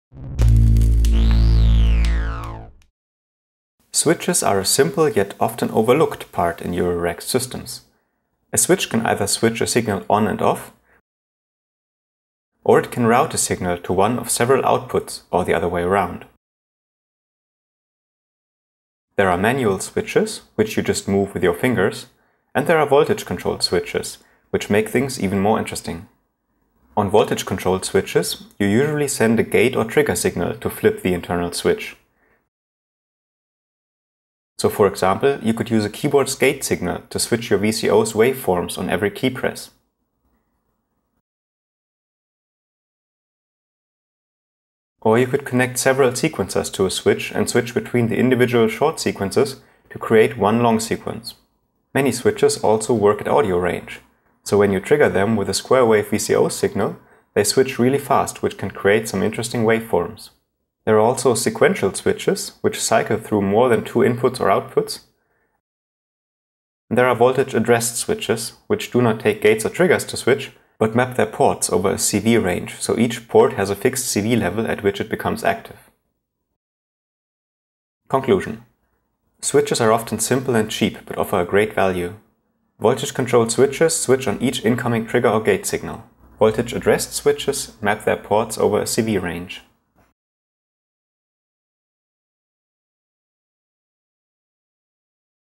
Modular Synth Basics #17: Switches
Eurorack switch modules are really cool, but often overlooked! Check out this video for more information about those little helpers :)